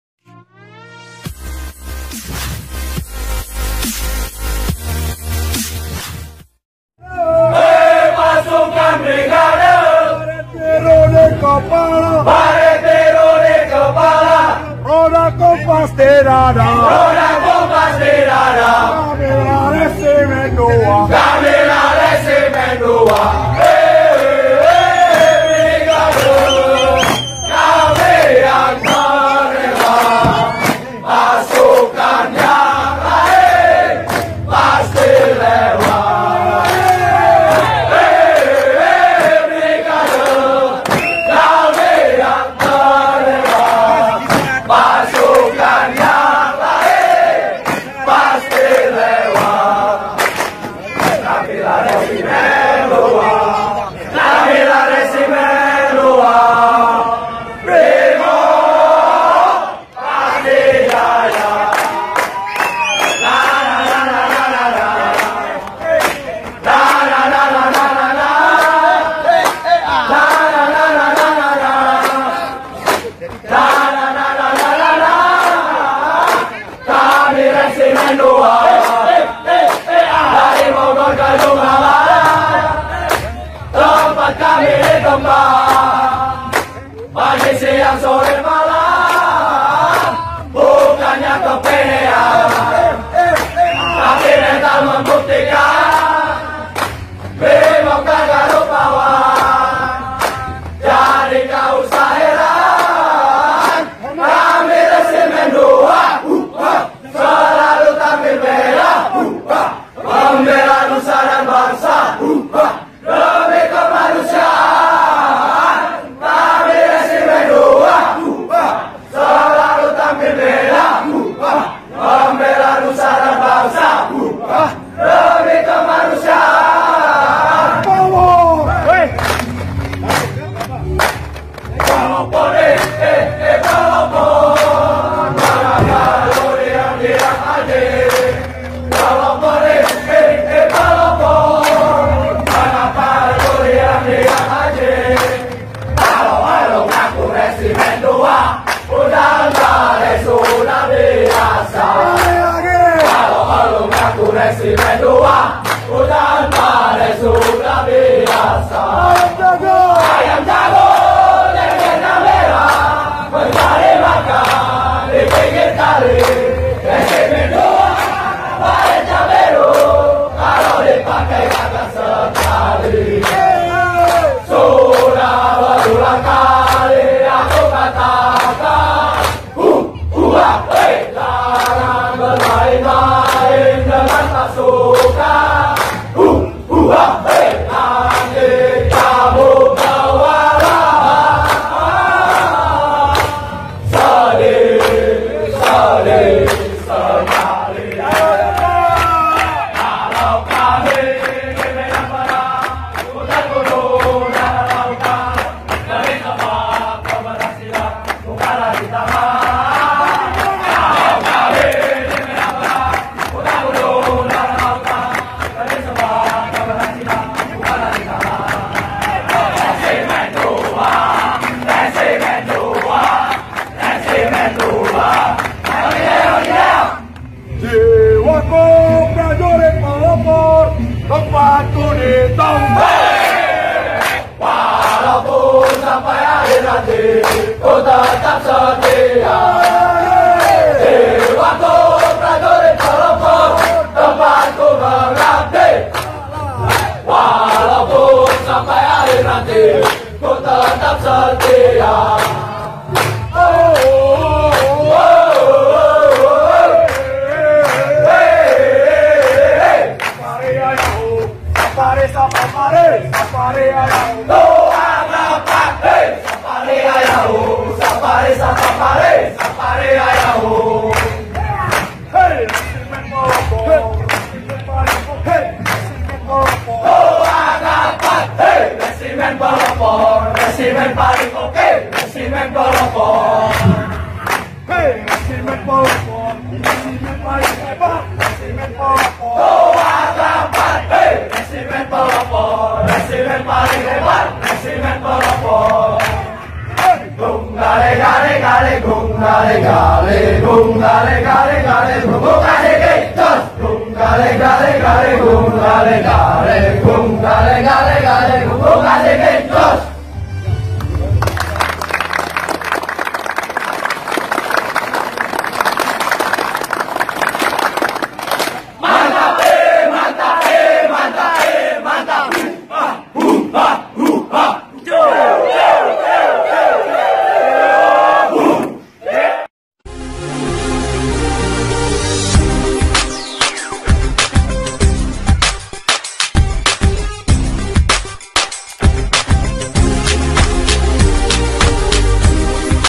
BLUR VIDEO SUARA YEL YEL BRIMOB #2
Video ini berisi blur video dengan suara yel yel pasukan Brimob